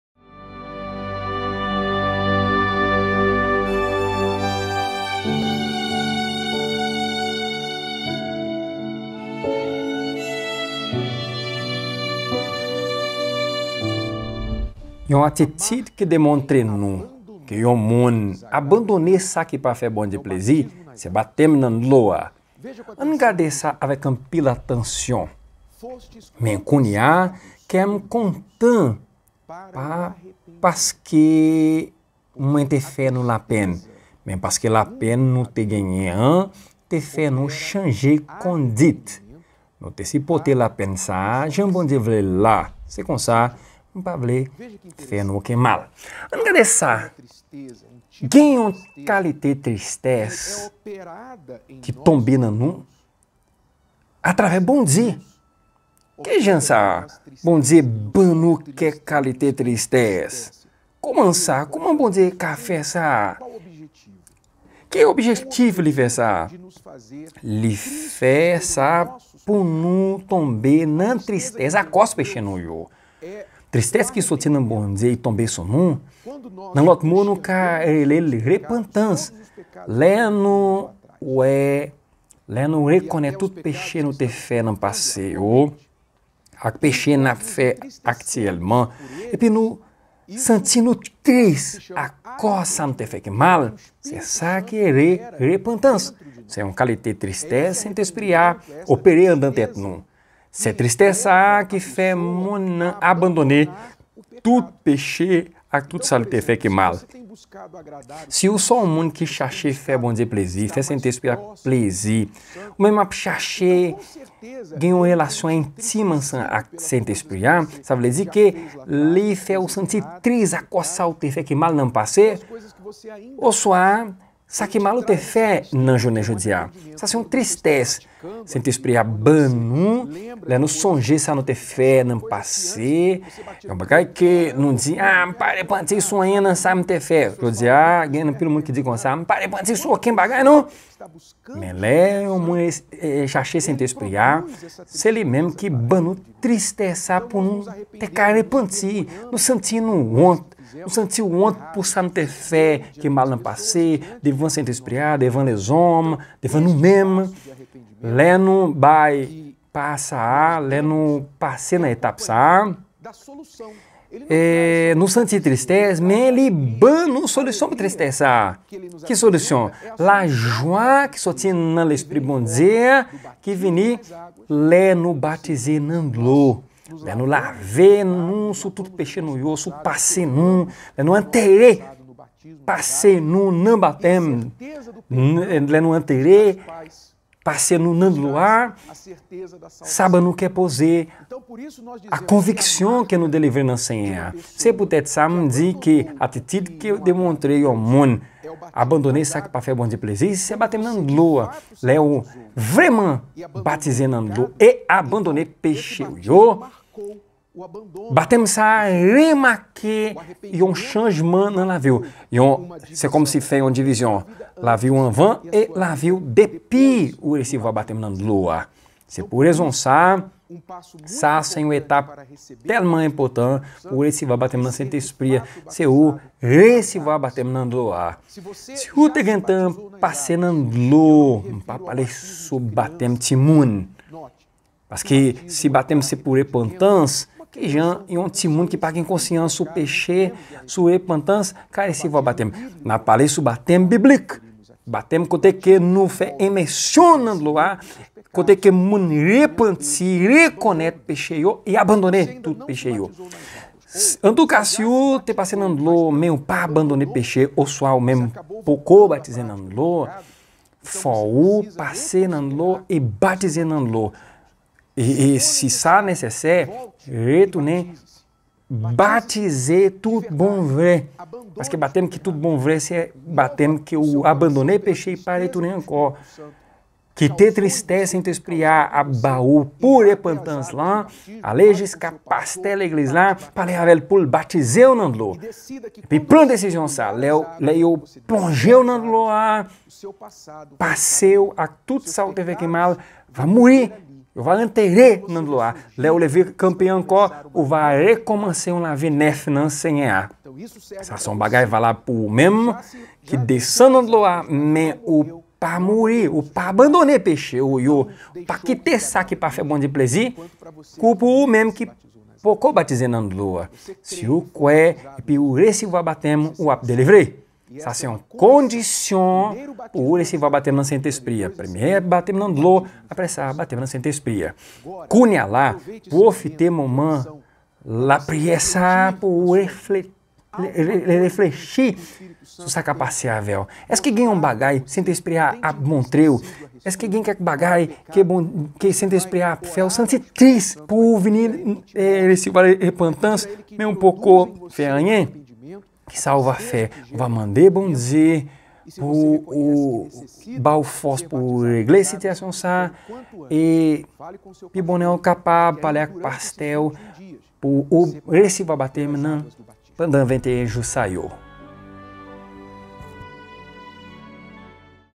VIDEYO 7/21 - 7° RESEVWA BATÈM NAN DLO A (Kijan pou ou resevwa Sentespri a nan 21 jou 🇭🇹
Gen yon kalite tristès ki se pwòp Bondye ki opere ansanm tèt nou. Tristès li fè nou santi se pou kondi nou a Repantans. Lè Sentespri a gen kondisyon pou fè sa, li fè nou pran youn nan desizyon ki pi enpòtan nan lavi nou: Resevwa Batèm nan dlo a...